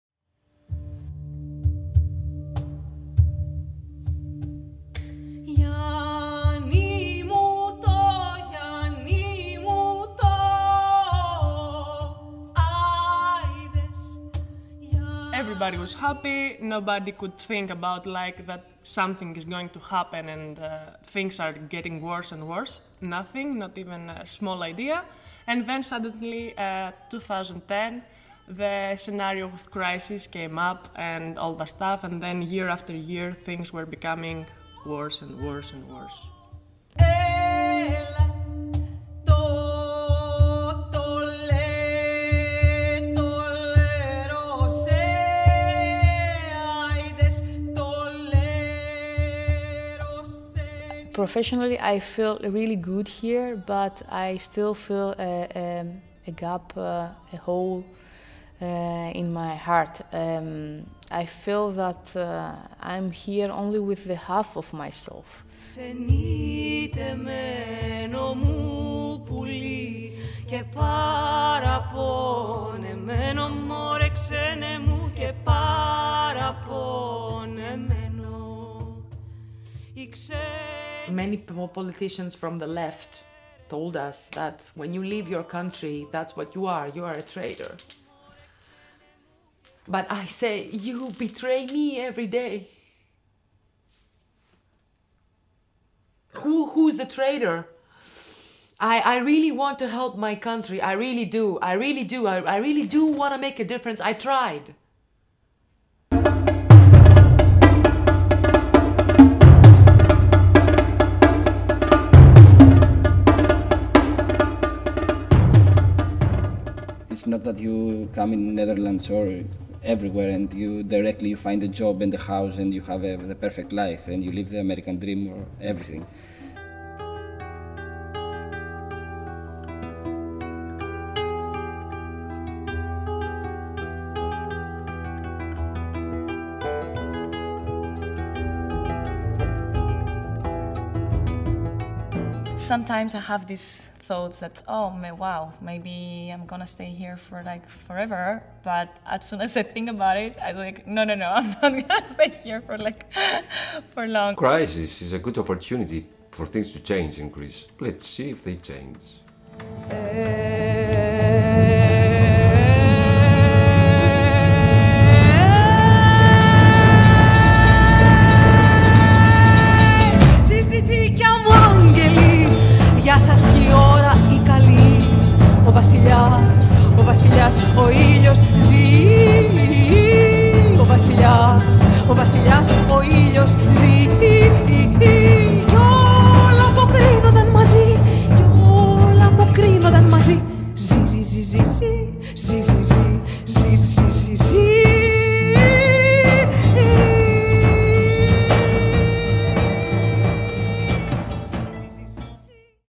Trailer Xenitia
Trailer of the premier of the show "Xenitia - a documentary concert about migration" on the 30th of April 2016 in Amsterdam.